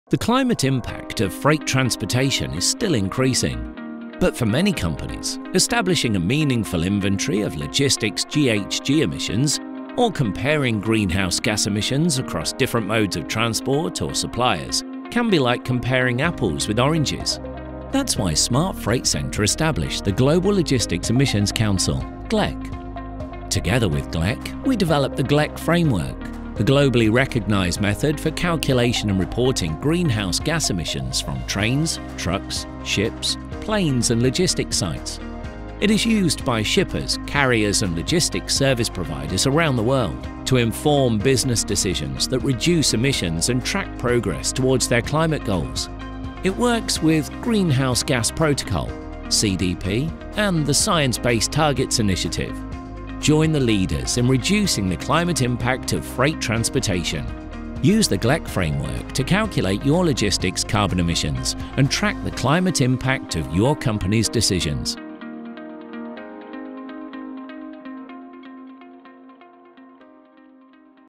Smart Freight Centre - What is the GLEC Framework?
The climate impact of freight transportation is still increasing.

But for many companies, establishing a meaningful inventory of logistics GHG emissions or comparing Greenhouse gas emissions across different modes of transport or suppliers can be like comparing apples with oranges.

That’s why Smart Freight Centre established the Global Logistics Emissions Council (GLEC) and developed the GLEC Framework: the globally recognized method for calculation and reporting Greenhouse Gas emissions from trains, trucks, ships, planes and logistics sites.